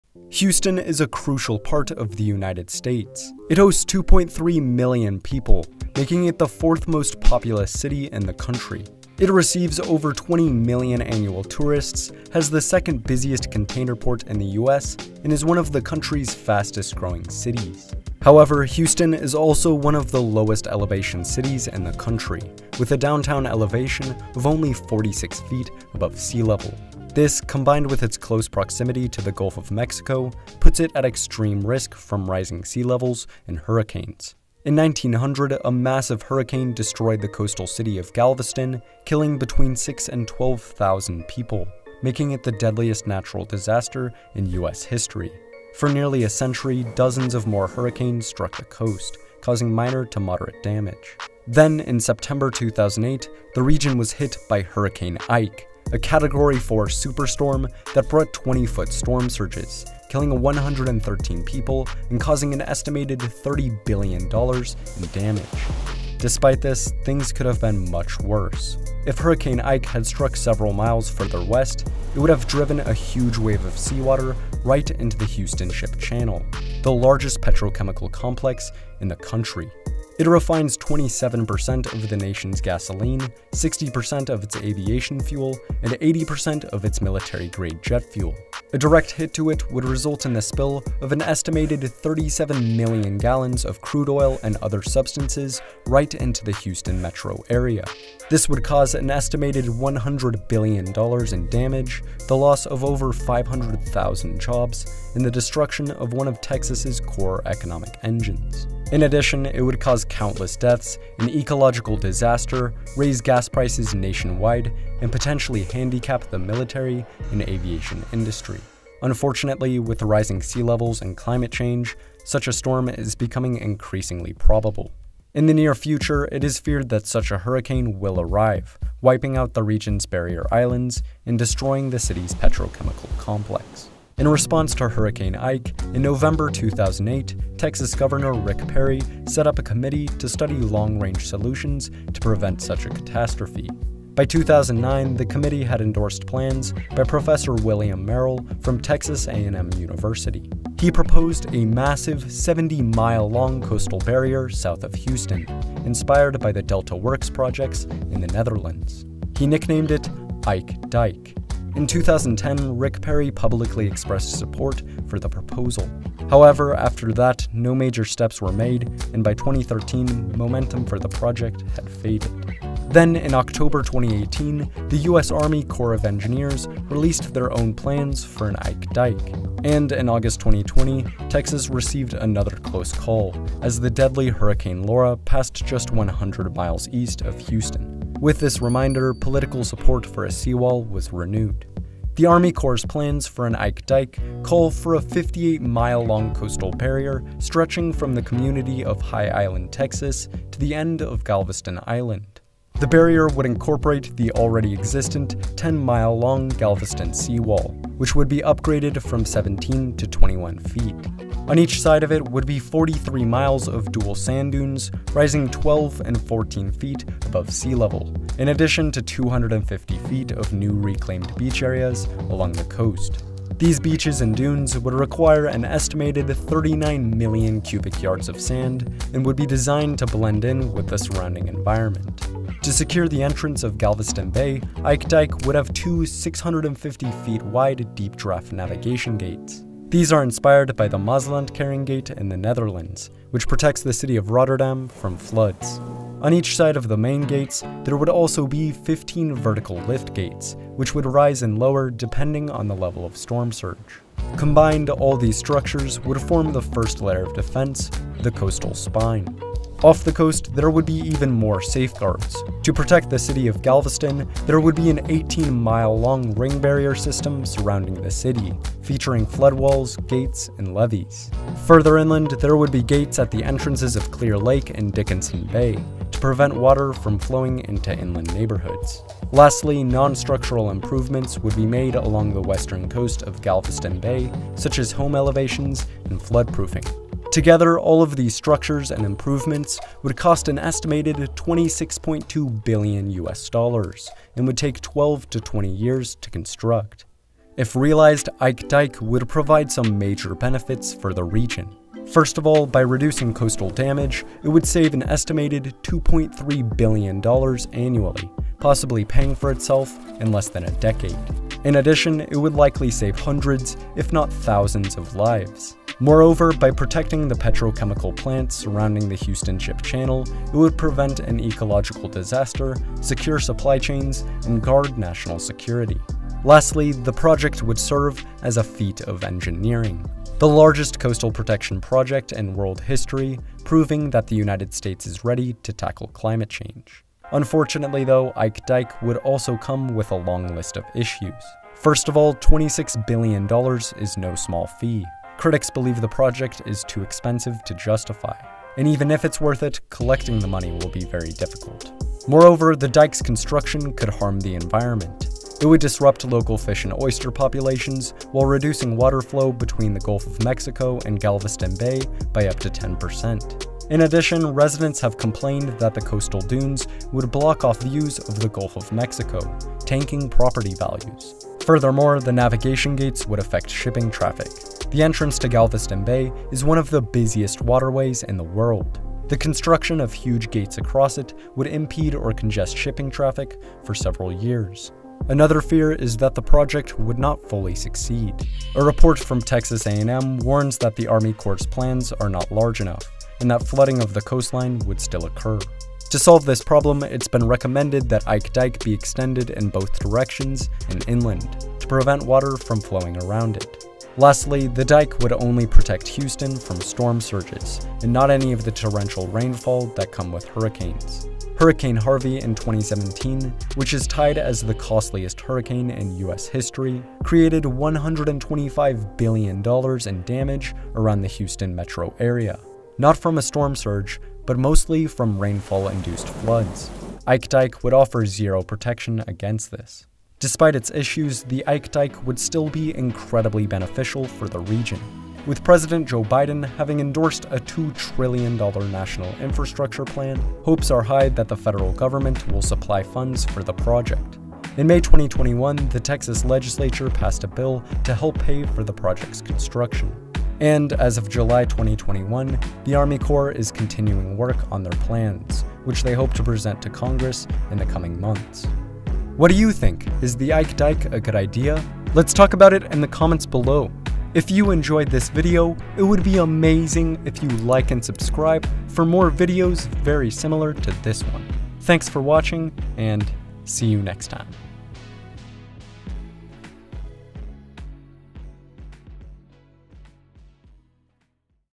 Texas’s Proposal for $26.2BN Seawall
Texas is considering constructing a $26.2 billion seawall south of Houston. Houston, the fourth most populous city in the USA, hosts the largest petrochemical complex in the country. It refines 27% of the nation’s gasoline and 60% of its aviation fuel. However, this complex is threatened by hurricanes and the storm surges they bring. If a hurricane directly hit the complex, it would result in an estimated $100 billion in damage. To help prevent this, professor William Merrill and the US Army Corps of Engineers have proposed a massive, 58 mile long seawall along the southern coast of Houston, nicknamed Ike Dike. The proposed seawall would protect national security and prevent a wildlife disaster. However, it would also be extremely expensive, could lower property values and disrupt local fish and oyster populations. As of 2021, the proposal is gaining momentum and is planned to be presented to Congress soon.

Thank you to the following sources!

Audio
Storyblocks

Graphics
Storyblocks
Google Earth